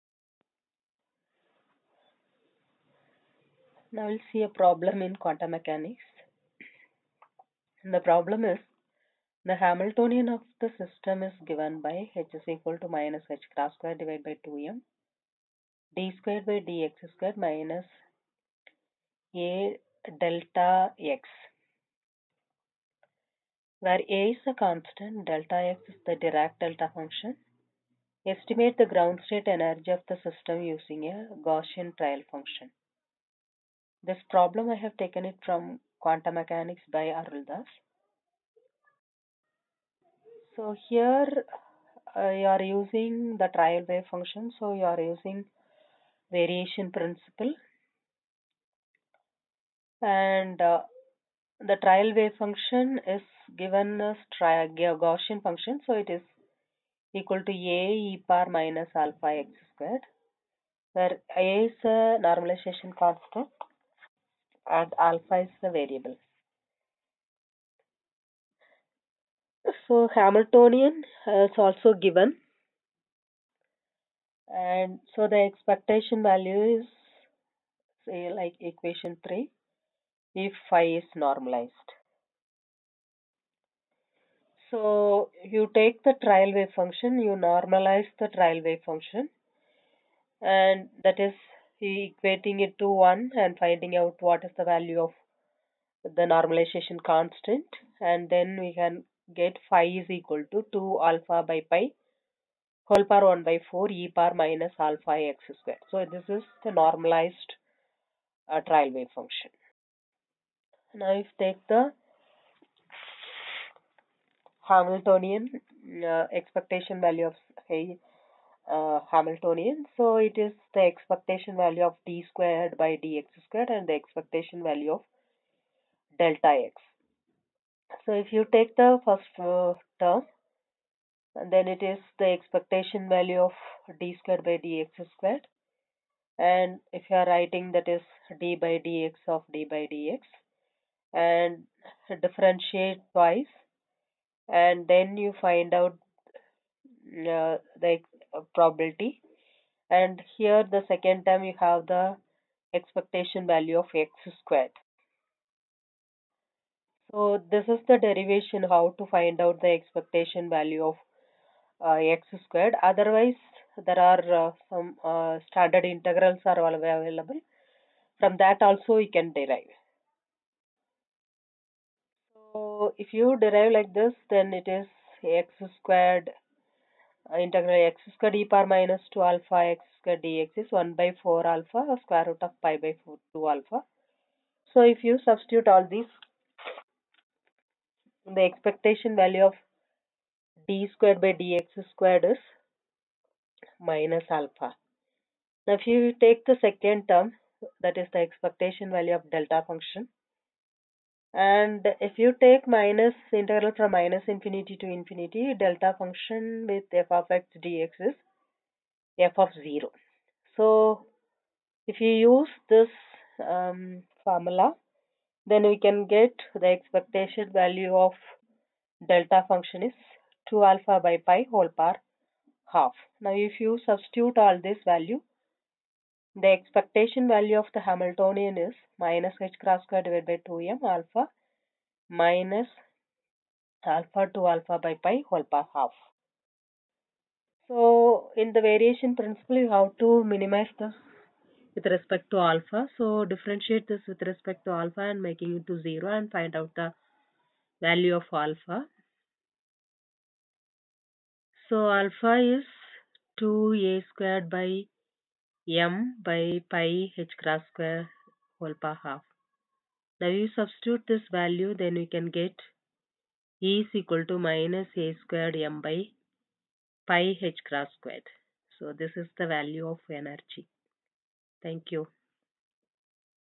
Estimate the ground state energy of the system using a Gaussian trial function-variation method
CSIR GATE Physical Science